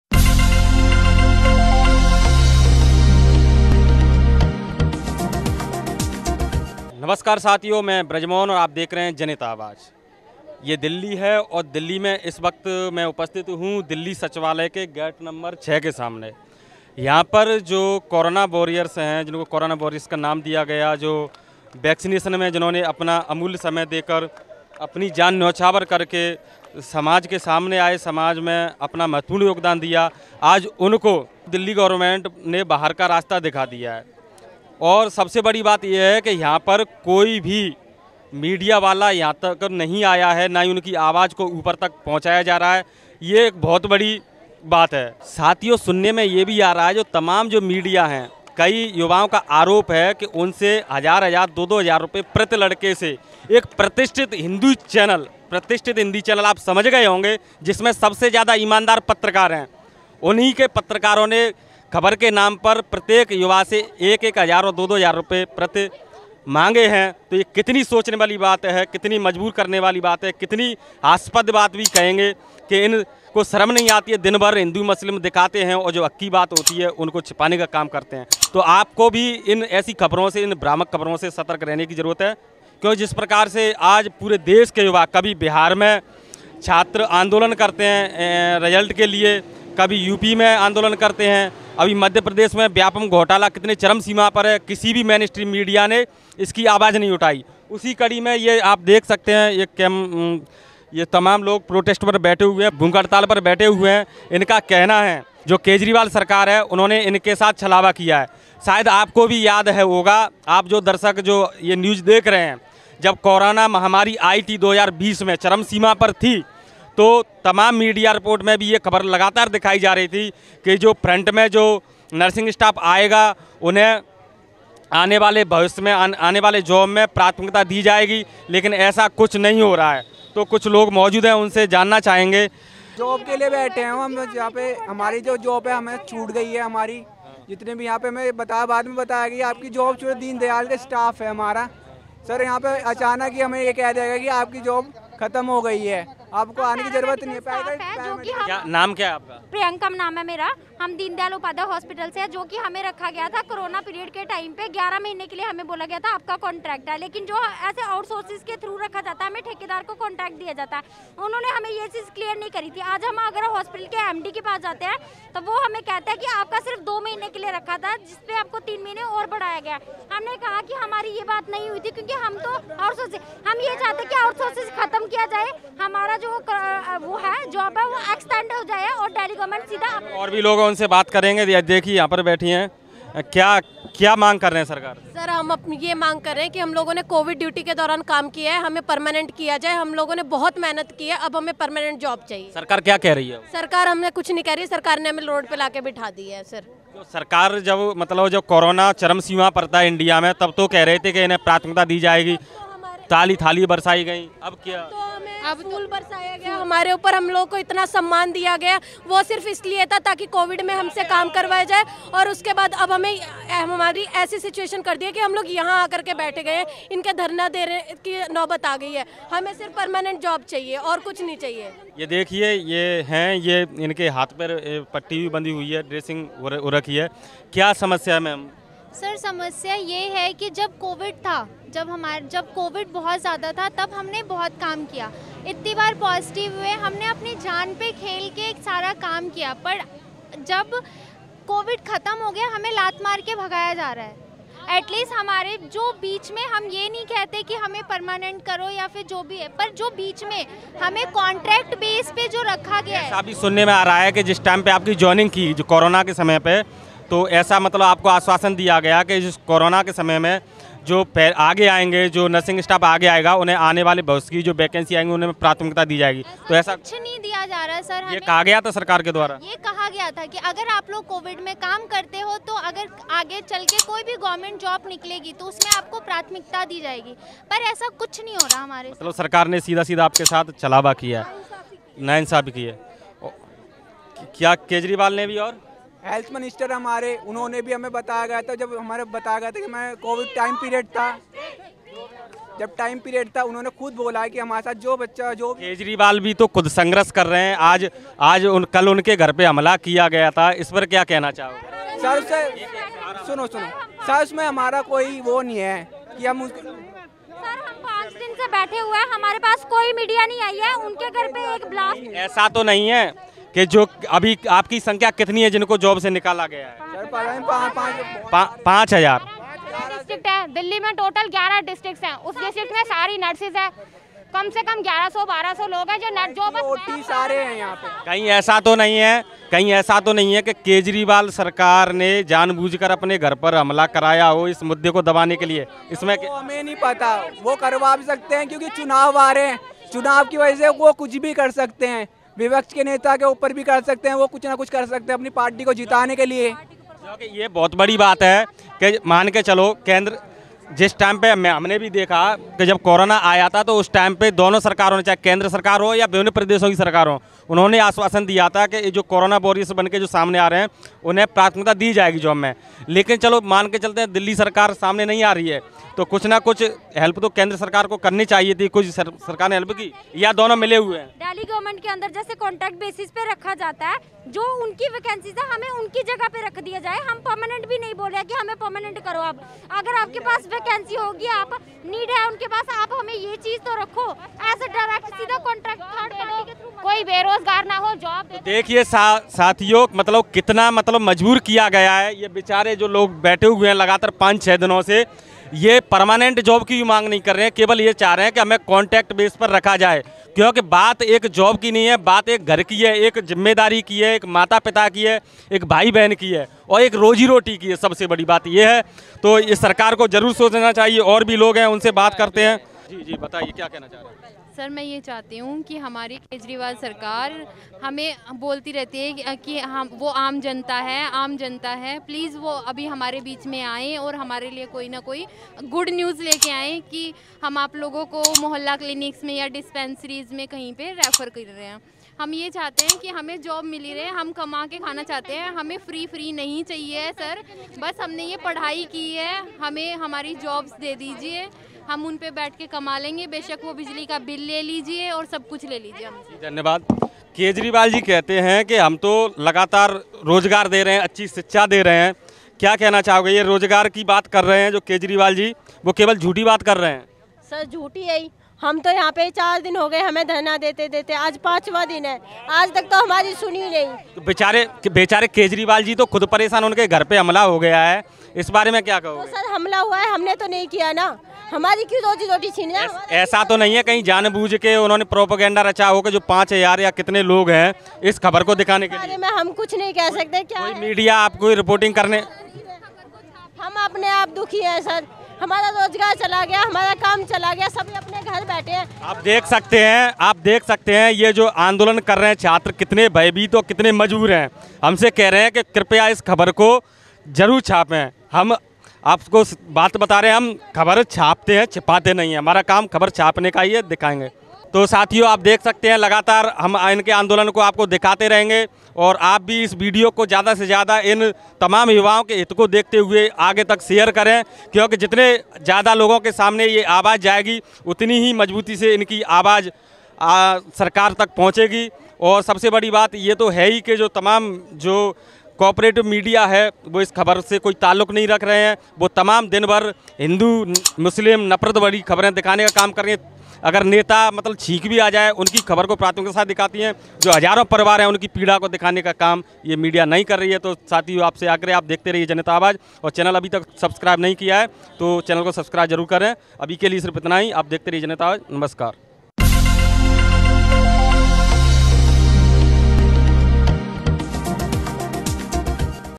कभी जिन पर फूल बरसाये आज उन Corona Warriors की कोई पीड़ा सुनने वाला नहीं | JANHIT AWAZ|GROUND REPORT
corona warriors speech,
10 lines on corona warriors,